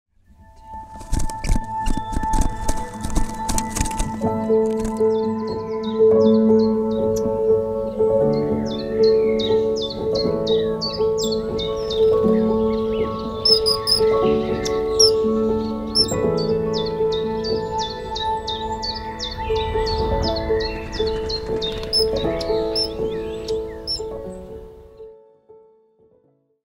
Kingfisher chick makes 1st flight after dad gives demo
Kingfisher chick flies for the first time, after a brief demonstration from dad